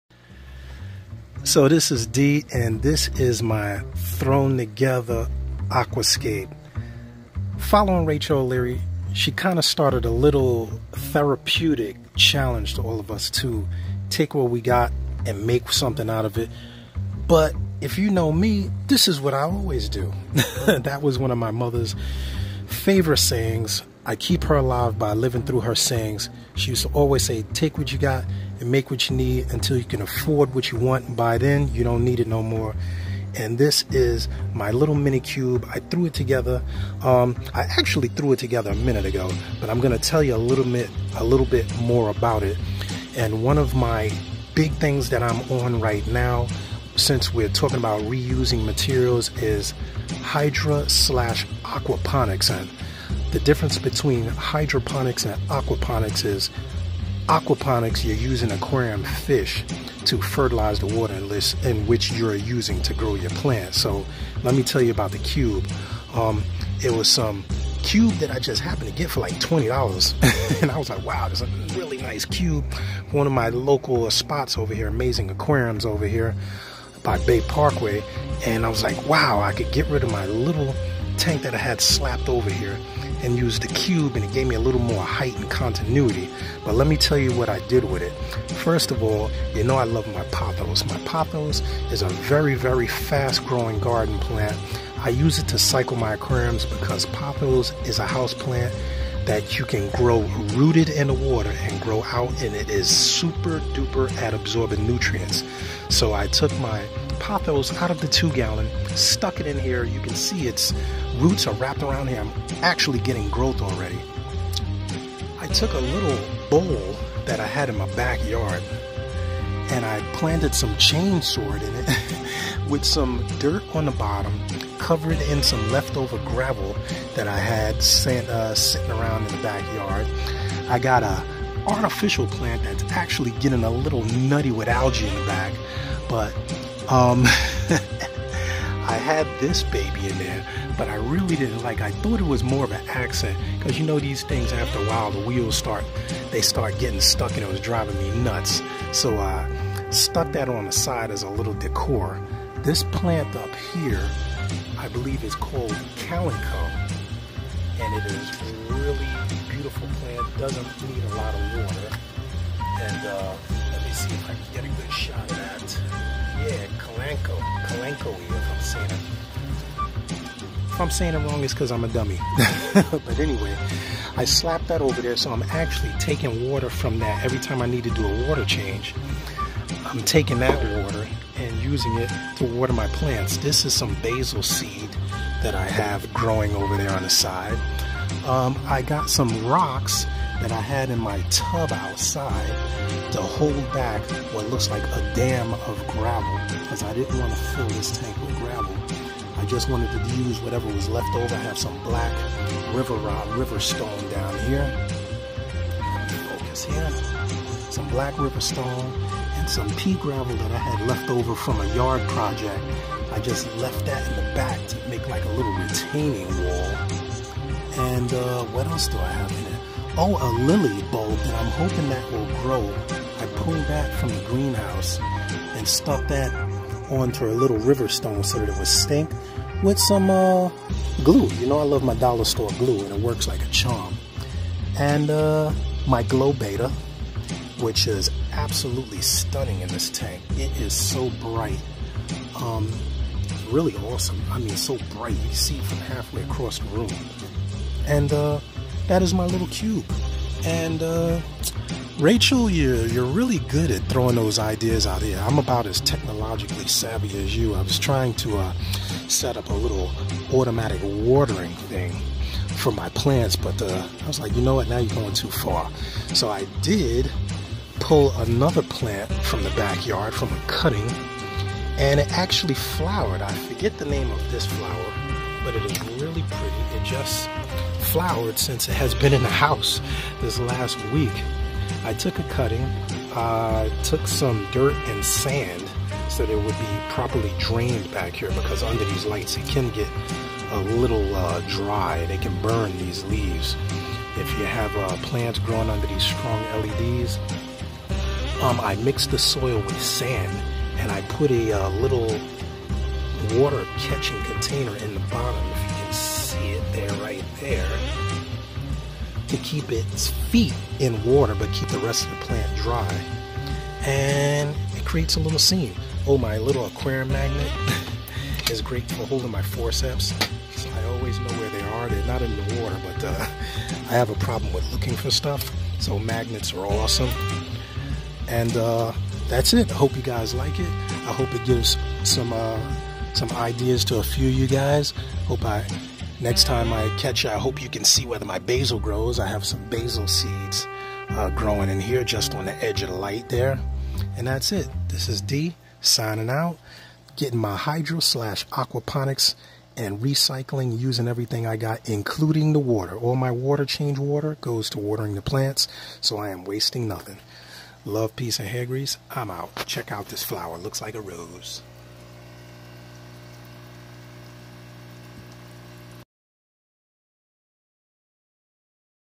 Scape from Scraps
He's my 6 gallon tank.   I love th6re challenge of creating from what you have already.   recycling at it's best